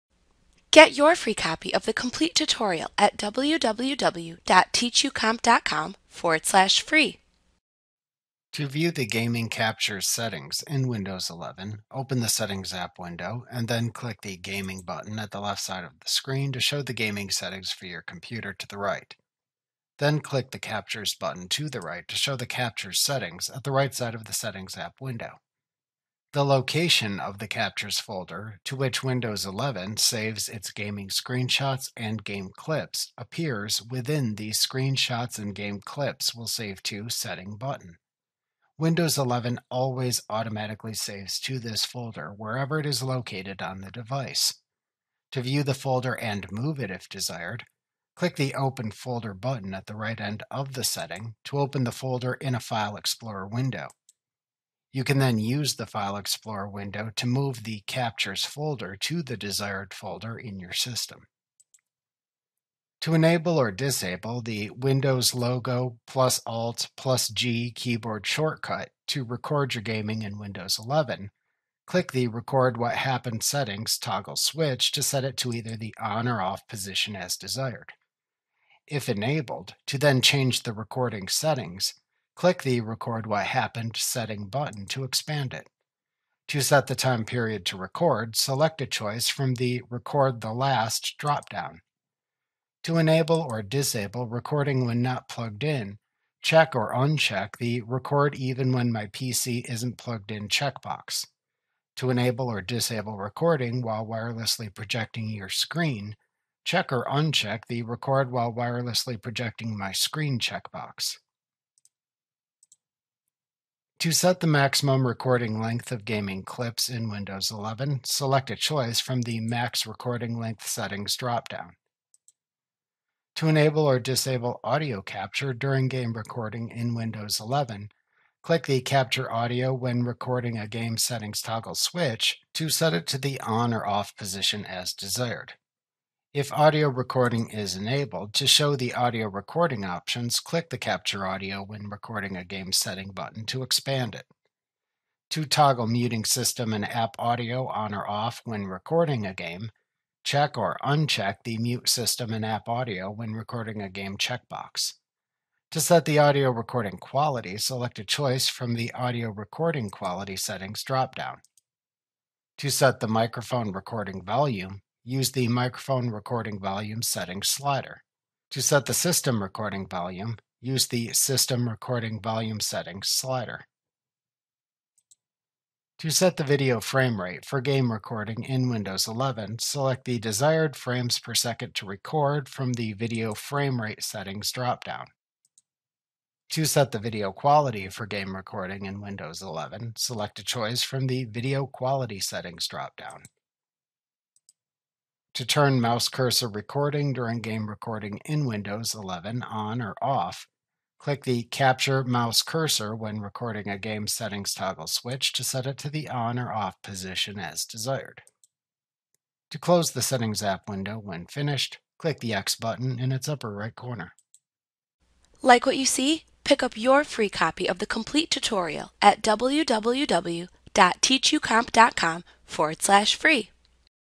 Learn How to Change the Captures Settings in Windows 11: A Training Tutorial
Learn how to change the captures settings in Windows 11 A clip from Mastering Windows Made Easy.